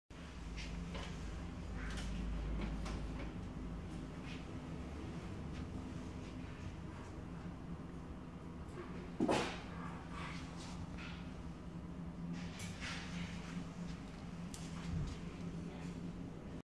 Сервал. 6 день после операции на мозге
абсцесс мозга. удаление